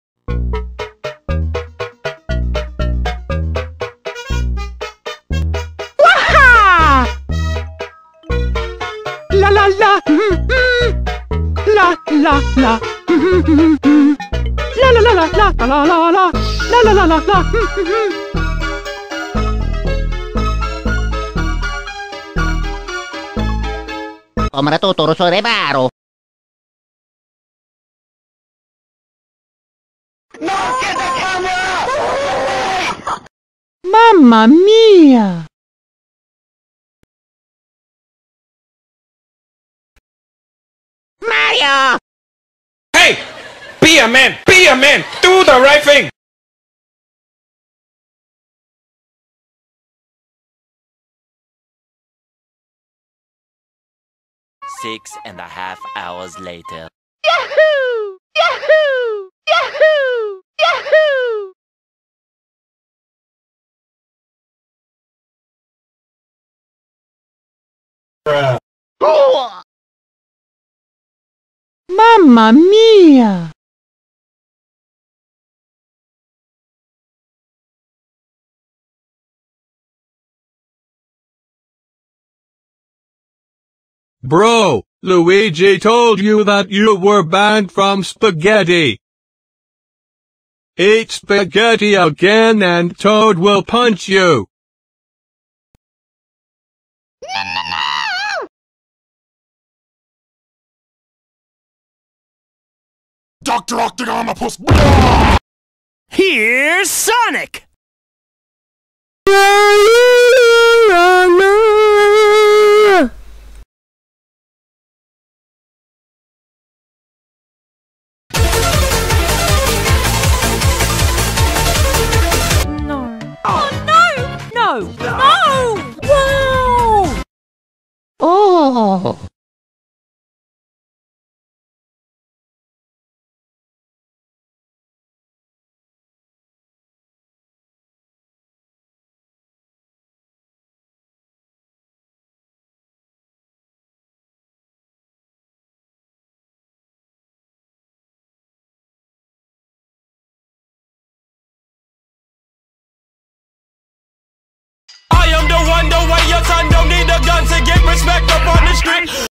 Mario gets banned from spaghetti
In this video, Mario will get banned and whenever he eats spaghetti he gets punched, what will Mario do about it